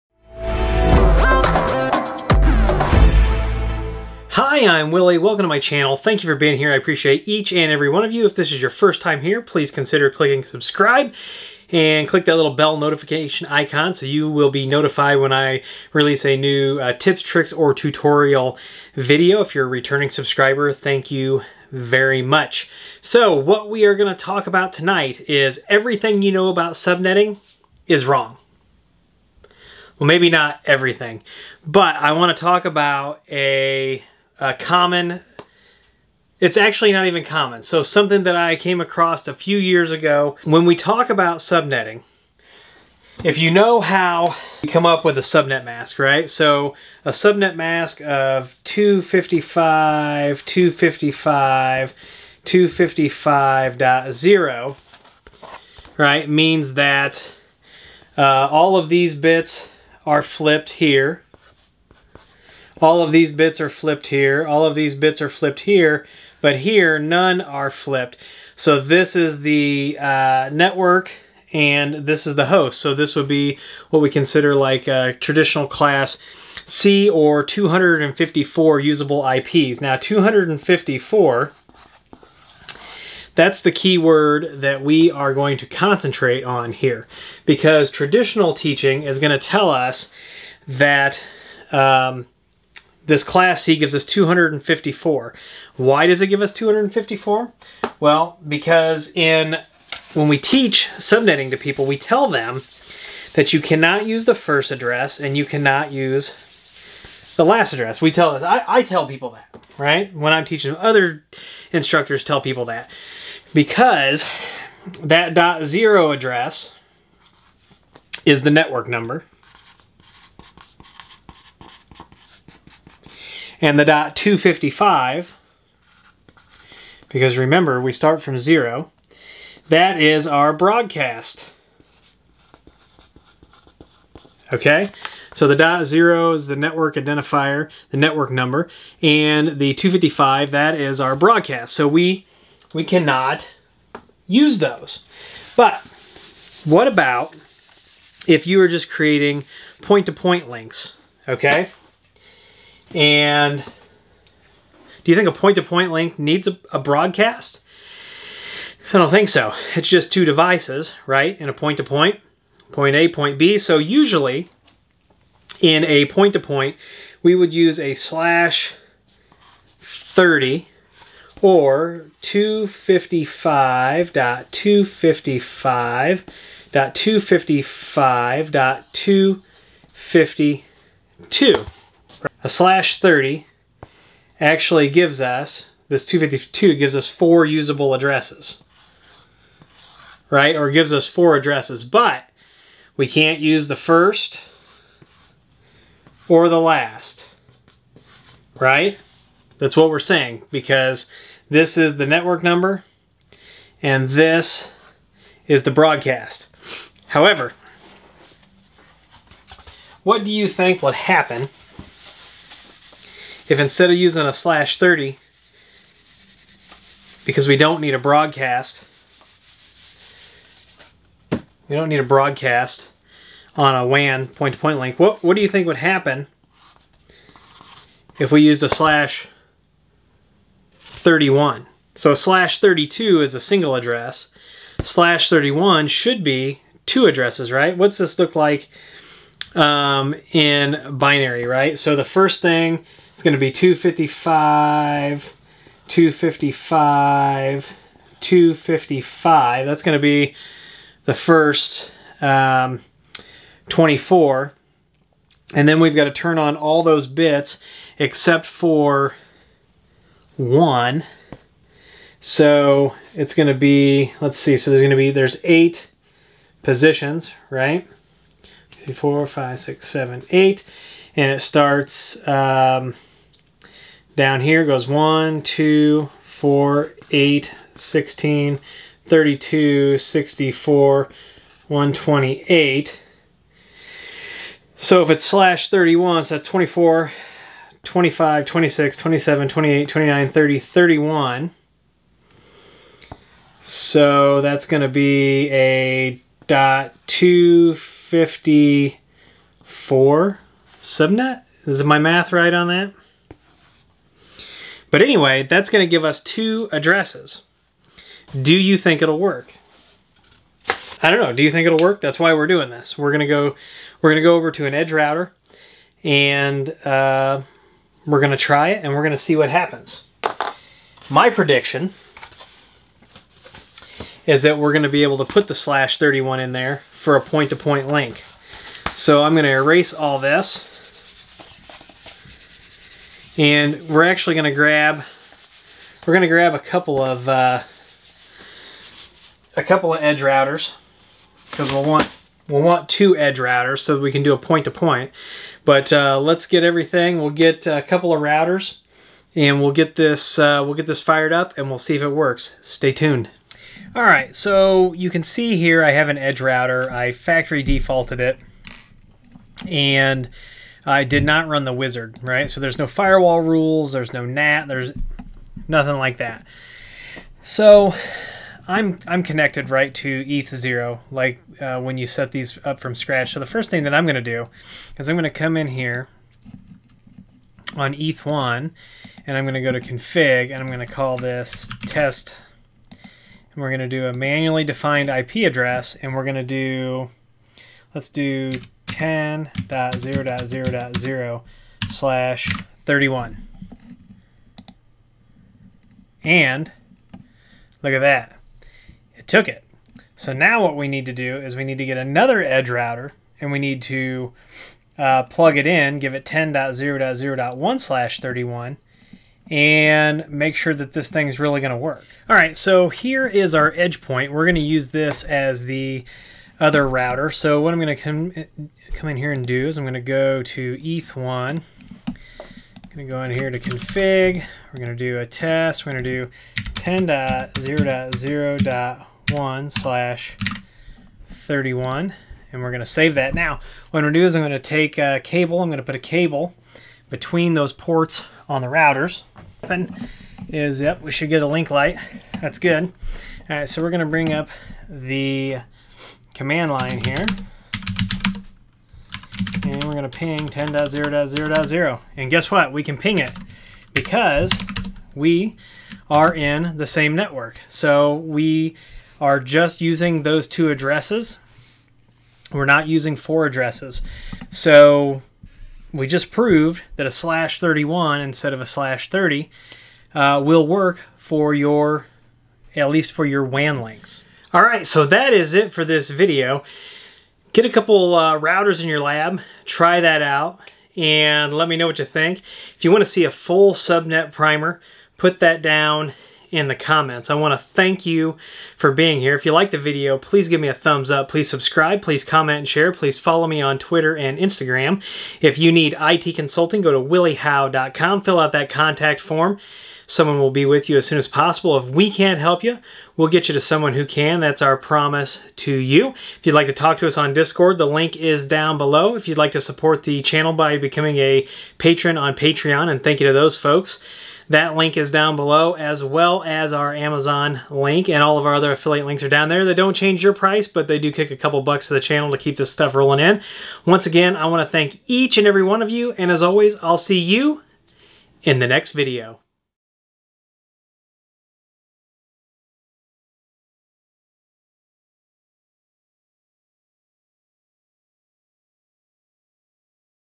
/31 Subnet - Everything You Know About Subnet Masks Is Wrong
Not everything!  But, do we need four addresses for a point to point?  Most modern day classes teach you that you can't use the first and the last addresses.

Have you been wasting IPs using a /30 subnet on your point to point WAN links?  Let's change that!

We support all Grandstream, Obihai, Polycom, Plantronics, Ubiquiti Networks, MikroTik, Extreme, Palo Alto, and more!